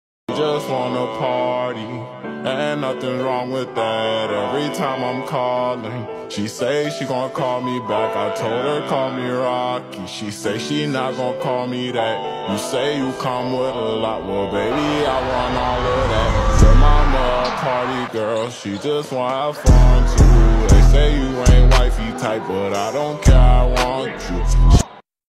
Back at Murray State Ja Was Him basketball college (1ne._.fineguy)
Step back in time and witness the early days of Ja Morant's rise to stardom in this incredible video of his time at Murray State! Watch as the future NBA phenom showcases his incredible talent and versatility as a player, dazzling with his dynamic drives to the basket, pinpoint passing, and clutch shooting. With his lightning-fast first step and incredible court vision, it's no wonder that Ja quickly became one of the most dominant players in college basketball. Whether you're a Grizzlies fan or simply love watching great basketball, this video is a must-see tribute to one of the sport's most exciting young talents. So sit back, press play, and get ready to relive Ja Morant's incredible journey from Murray State to the NBA!